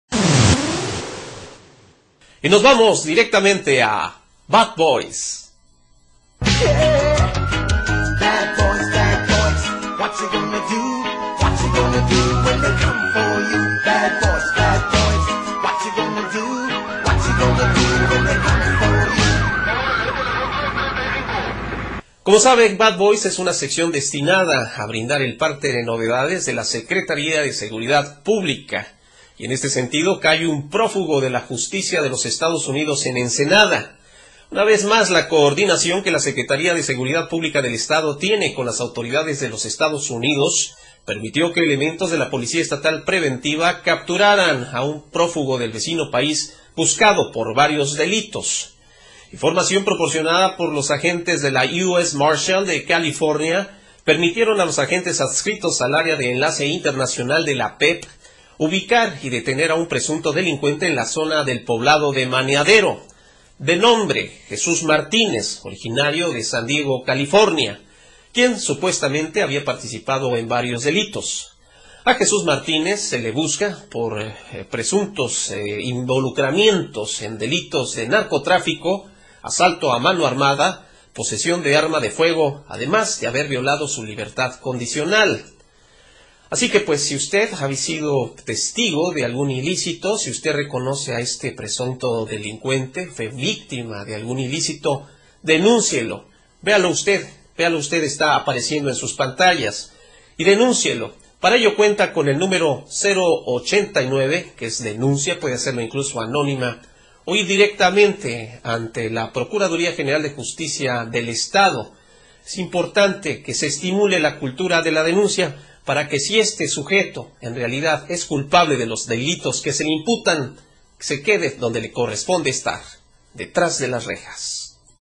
CAE PROFUGO DE LOS ESTADOS UNIDOS 22 abril 2010
Hector Polo produce y conduce el noticiero de "NOTIVISION" desde México.